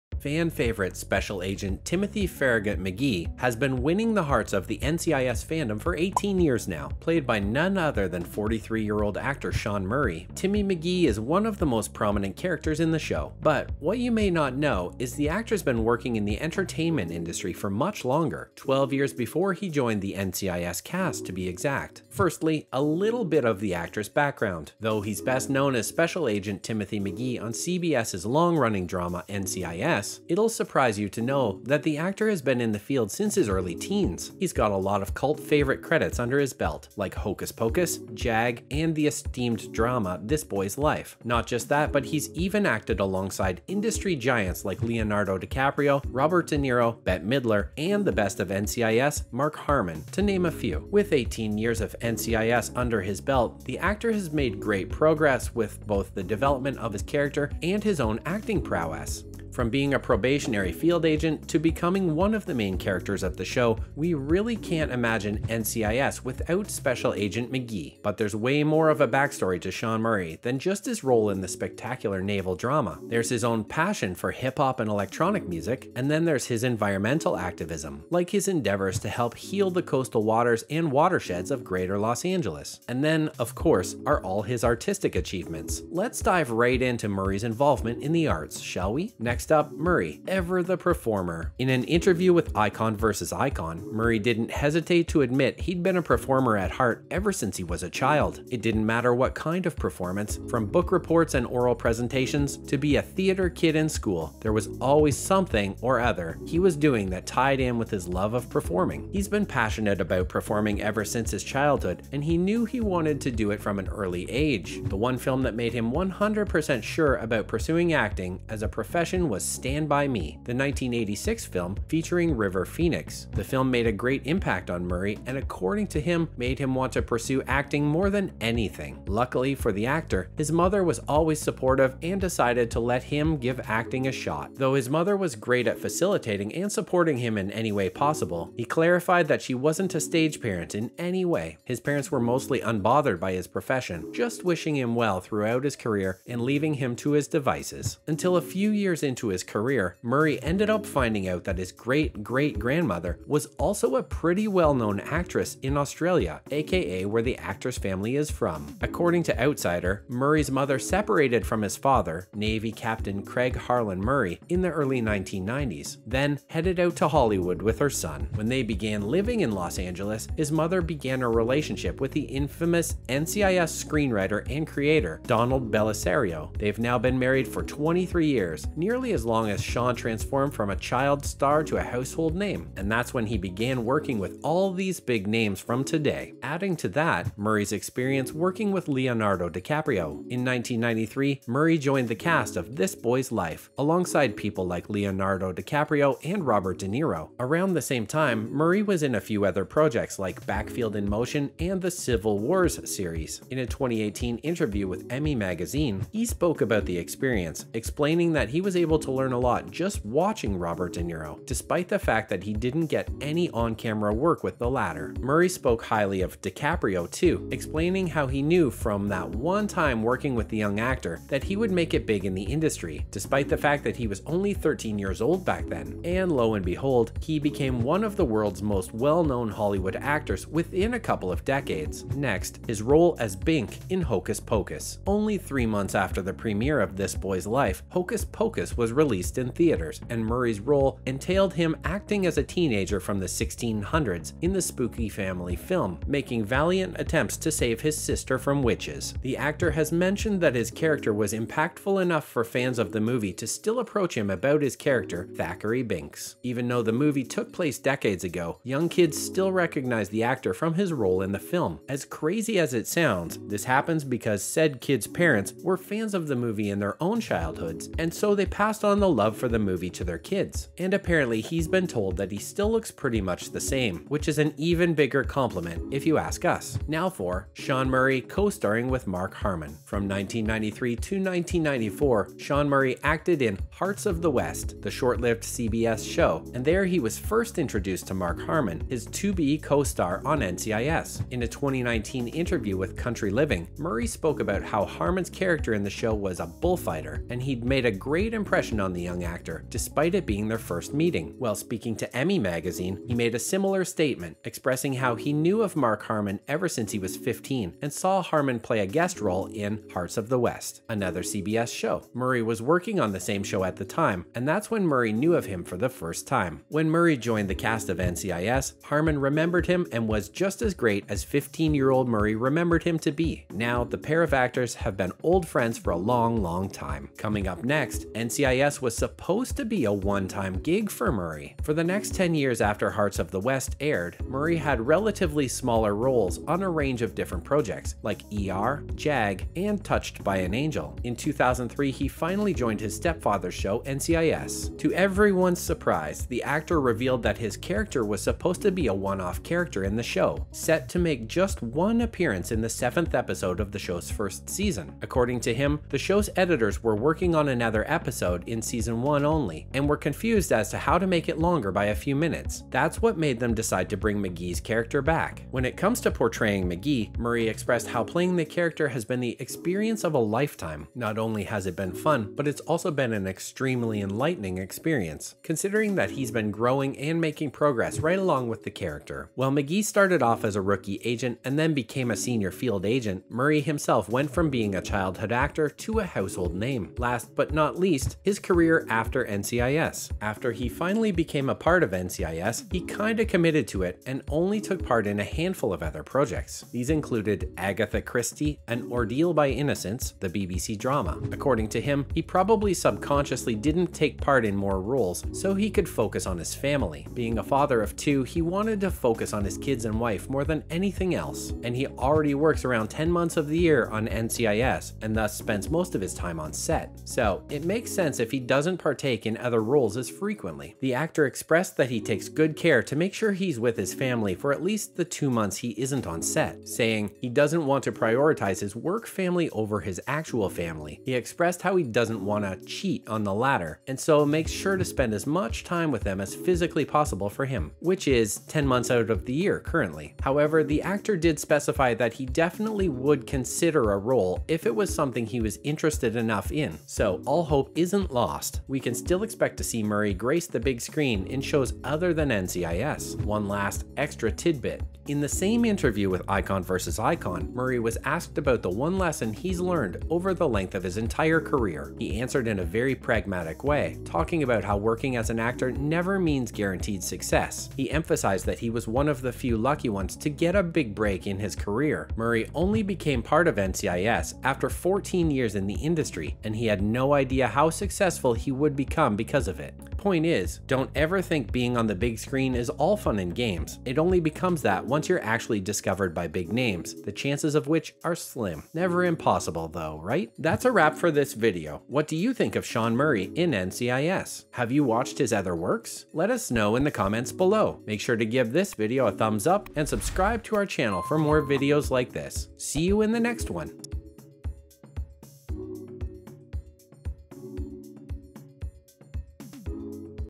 Sean Murray Transformation Then & Now REVEALED..
Welcome back to Film Recall. For the channel today, we will be featuring The Transformation Of Sean Murray From Childhood To NCIS. One of the favorites in the TV series NCIS is none other than Special Agent Timothy Farragut “Tim” McGee.

Tim is played by actor Sean Murray who has been in NCIS for almost two decades now. While it is true that Sean Murray became popular in NCIS, many fans do not know that he has been in showbusiness long before his days in NCIS. Here’s The Transformation Of Sean Murray From Childhood To NCIS.